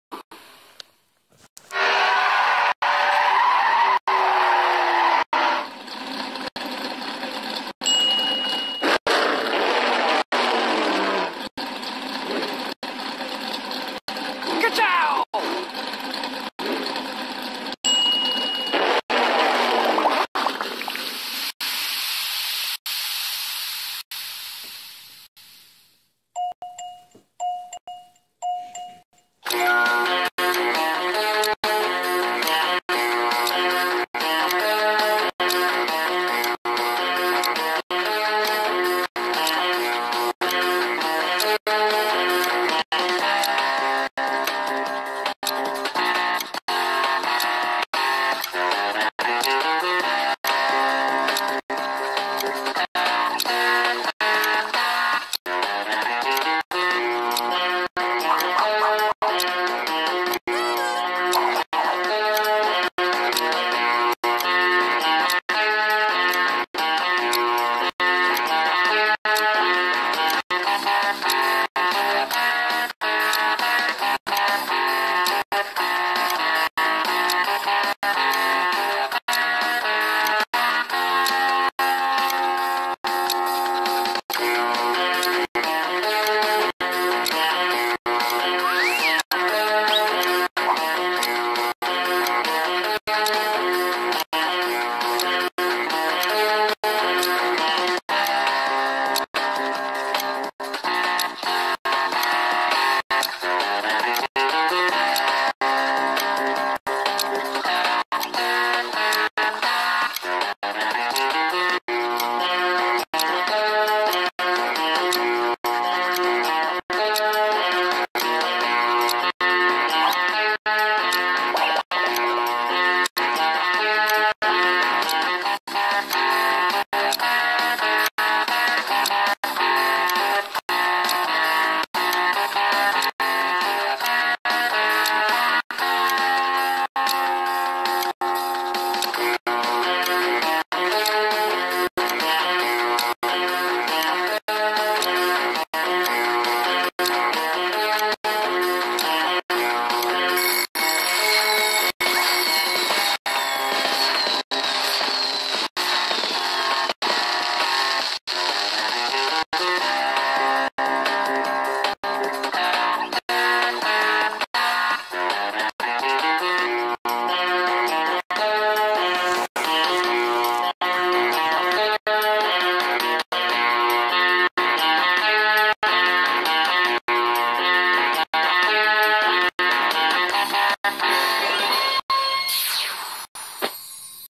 Magic Timer 2 Minute Brushing Video with Cars (8) - Lightning McQueen World Sticker
Oral-B Magic Timer App for 2 Minute Tooth Brushing
This video slowly reveal the sticker of kids' favorite characters.
It runs for 2 minutes with 4 breaks every 30 seconds to change the tooth section to be brushed.

This is perfect for children to continue brushing to 2 minutes.

Theme: Disney Cars
Opening: Lightning McQueen
Sticker: World Poster